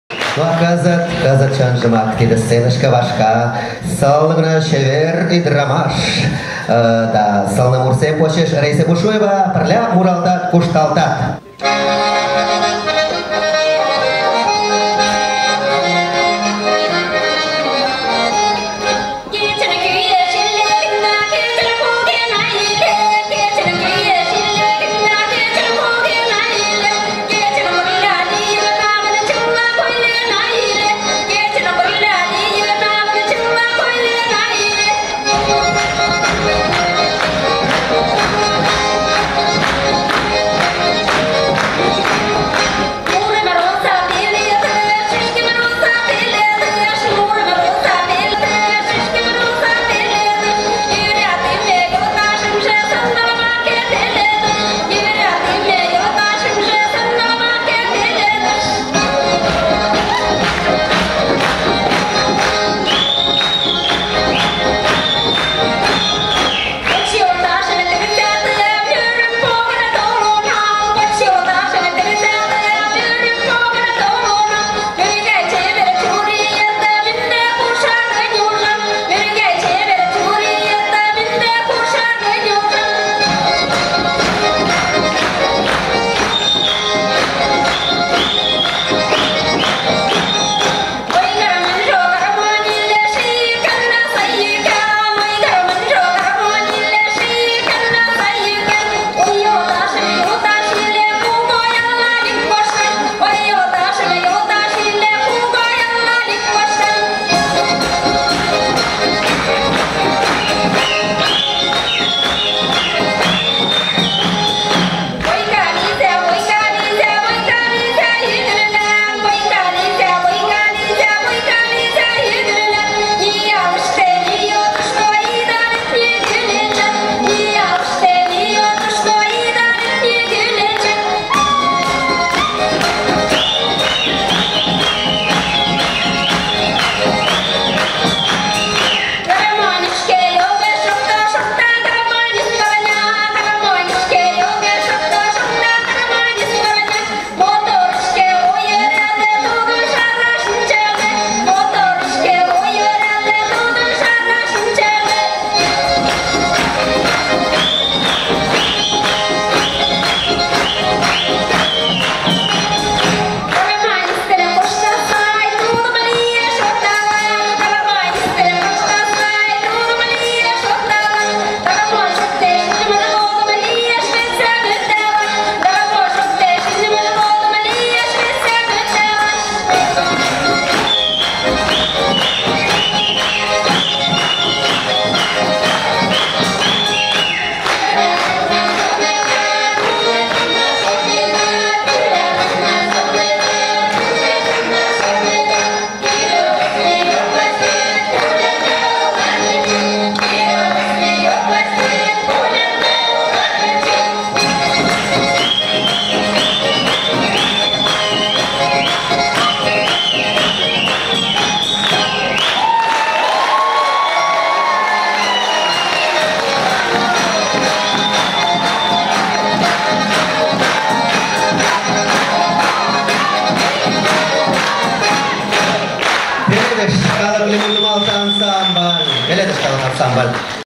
Раисия Бушуева да "ПЕЛЕДЫШ" калык ансамбль (Вятские Поляны) - Марий такмак-влак
"Киров вел - мыйын шочмо вер" концерт-проект. 2020 ий 7 март. Йошкар-Ола.
Кумылан-влак тичмаш концертым налын кертыда. Видео-дискат уло.